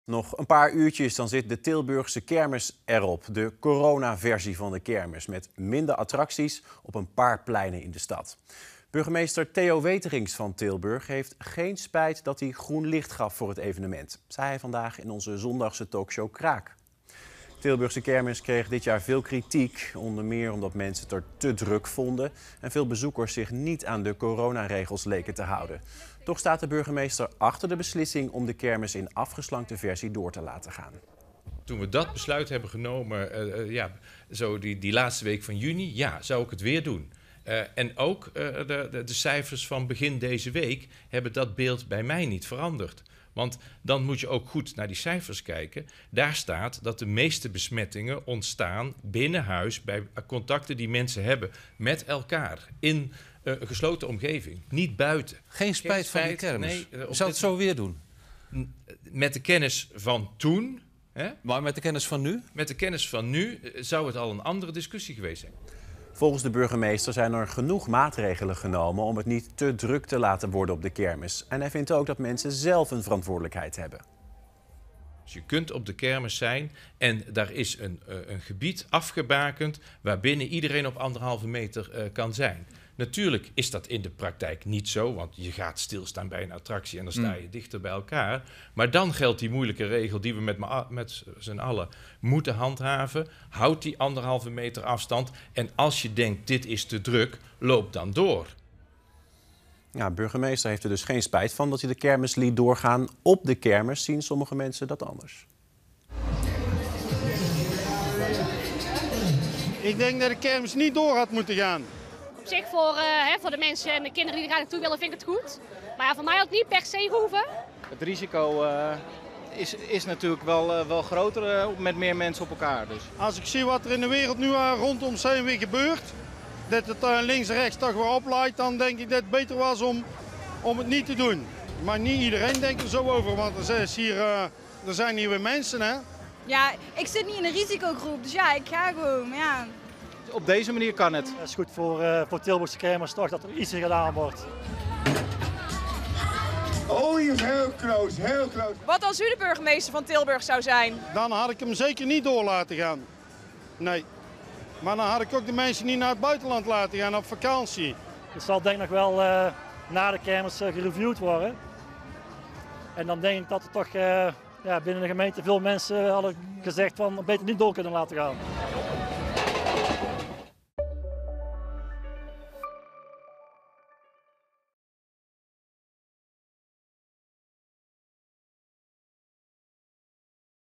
Bezoekers blikken terug op Tilburgse Kermis | Omroep Brabant
De meningen over het door laten gaan van de Tilburgse kermis zijn verdeeld.  Al het Brabantse nieuws volgen?